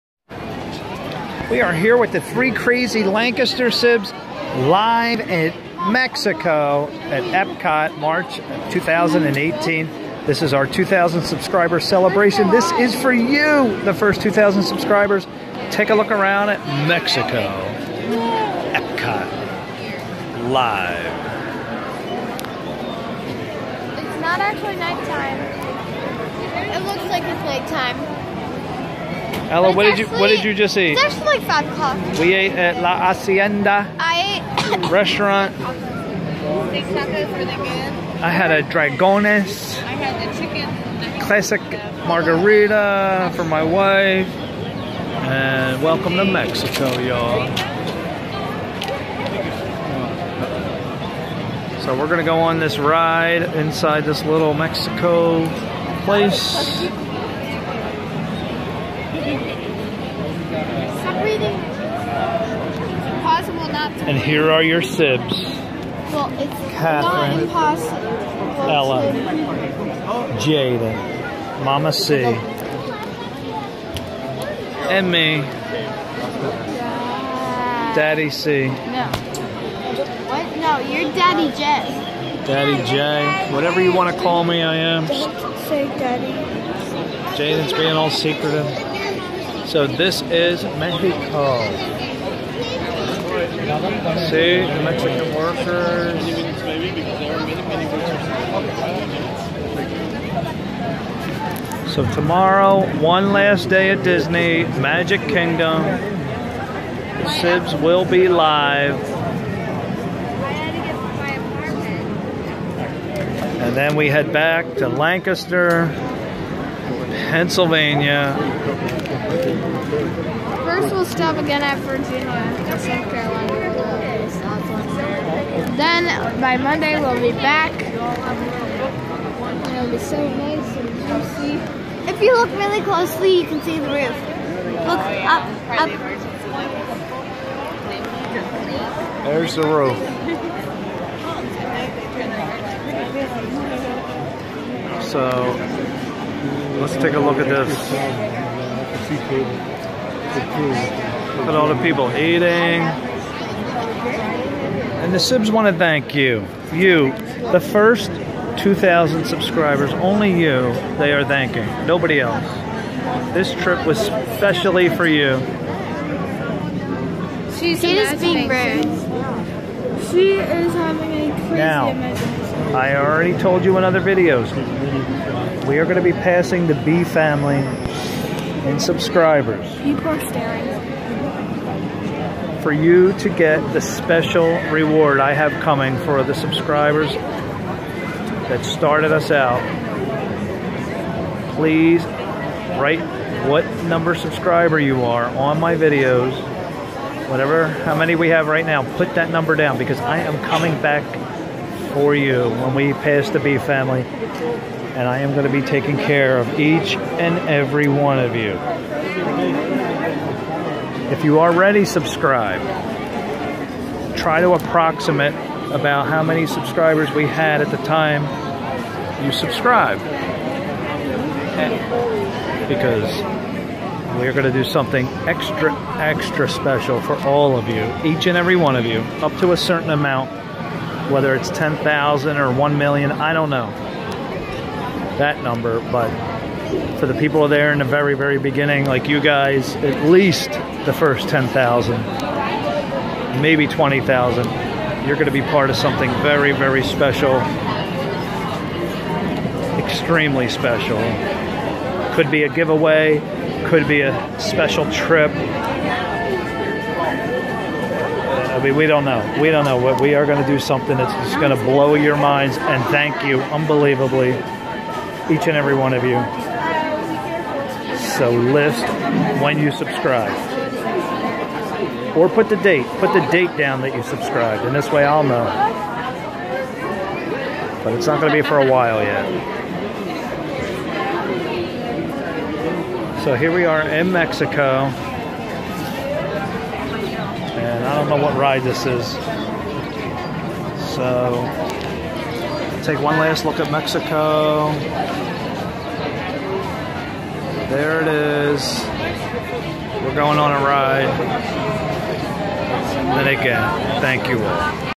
Mexico - Epcot - March 2018
The three crazy Lancaster saves are live in this video at Mexico in Epcot Disney World filming line right before they get on and ride in Mexico. Again this is for First 2000 subscribers as our way of saying thank you! We hope you enjoy our 2000 Subscribers celebration trip to Walt Disney World !!!